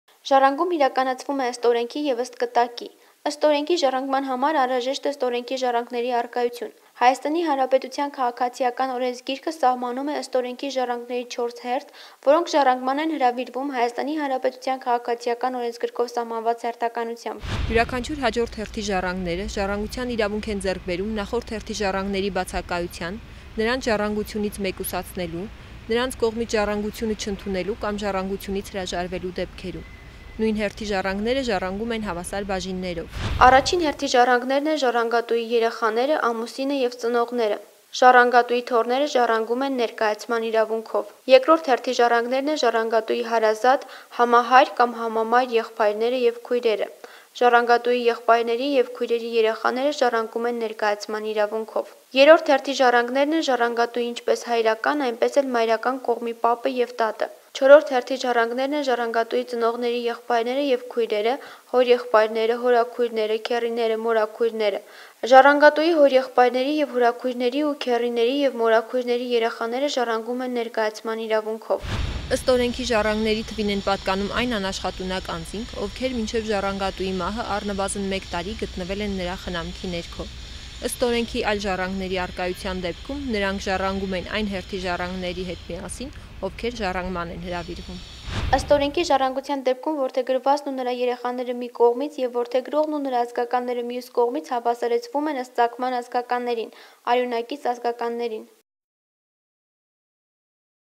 Ի՞նչ հերթականությամբ են ժառանգում
Իրազեկ քաղաքացի նախագծի 105-րդ տեսանյութի խորագիրն է՝ Ի՞նչ հերթականությամբ են ժառանգում ըստ օրենքի ժառանգները, փորձագետը՝ «Հայաստանն ընդդեմ կոռուպցիայի» ծրագրի փաստաբան Արփինե Եղիկյանը: